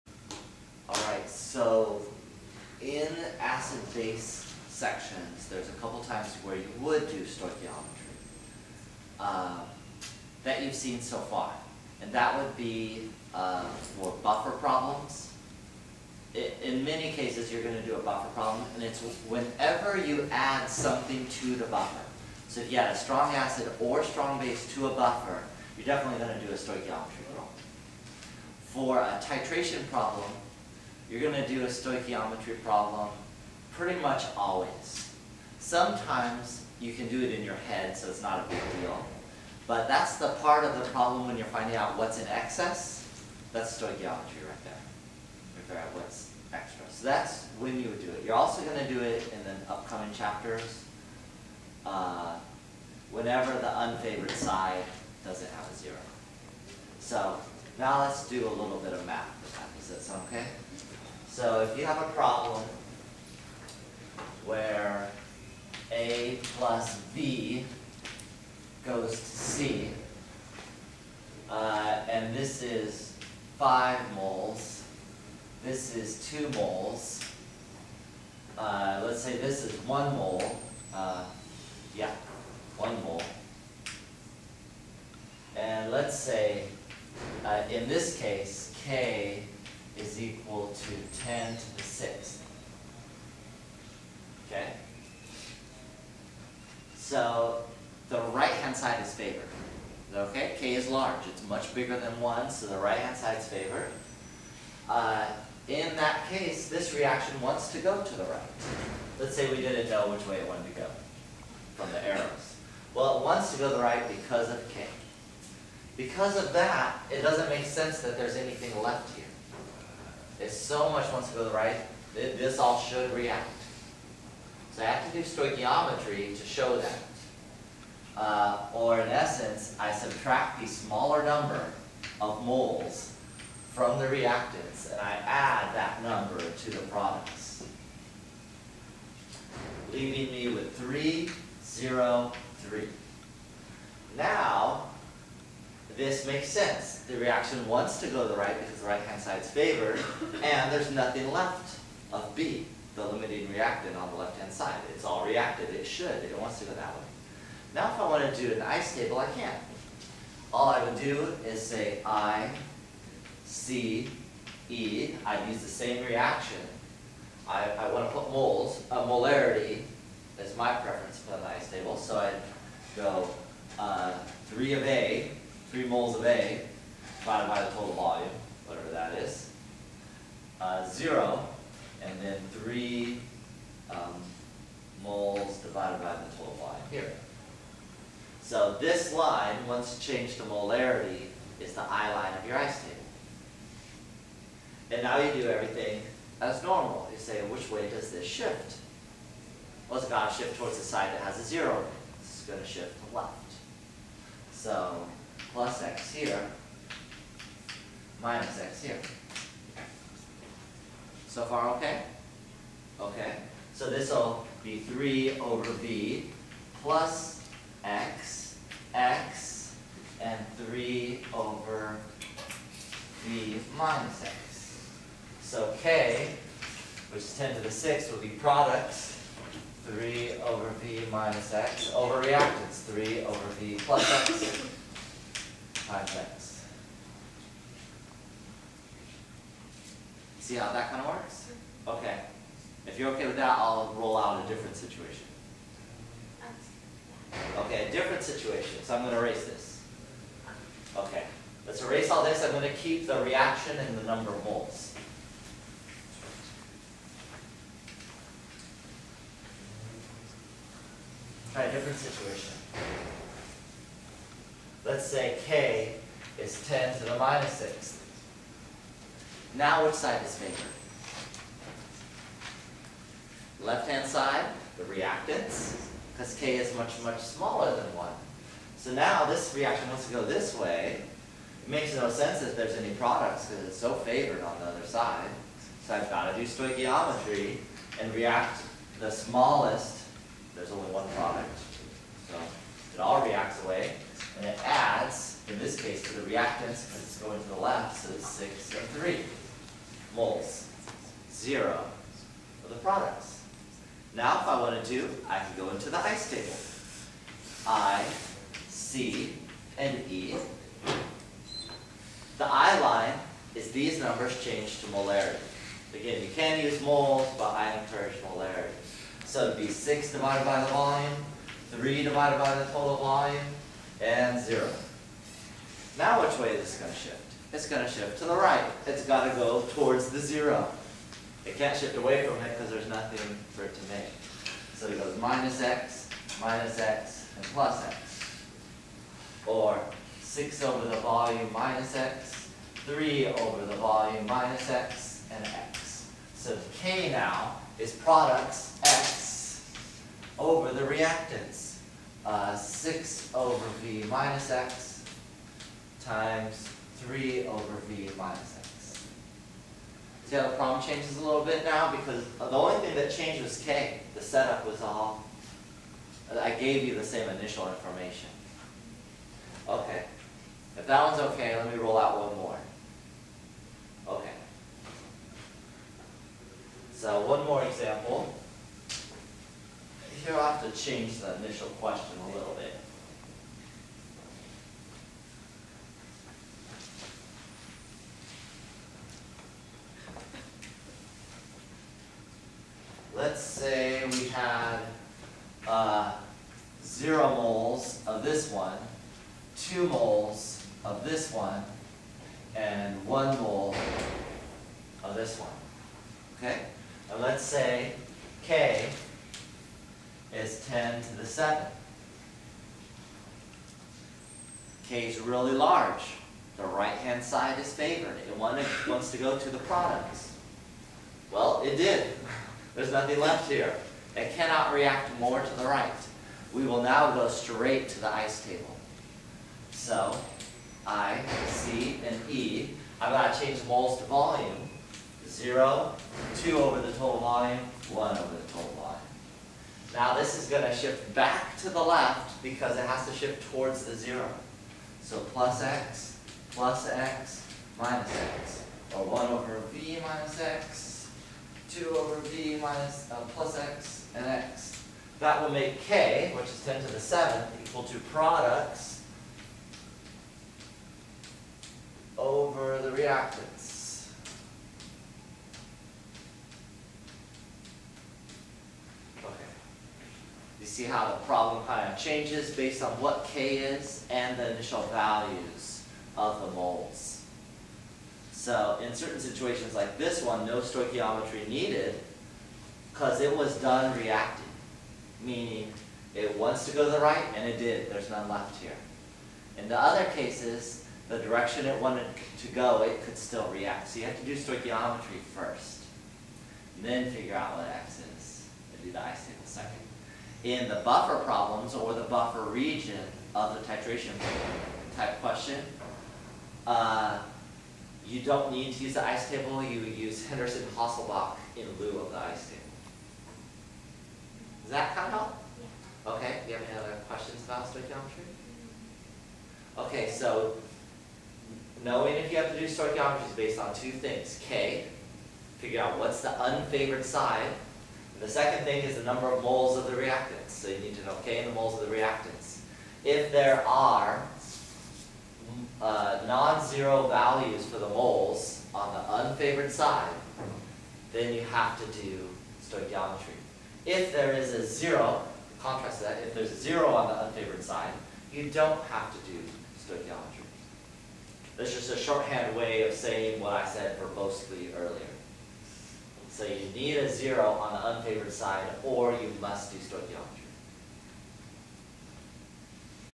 Stoichiometry Concept.mpg
When doing equilibrium problems, when are stoichiometry tables necessary?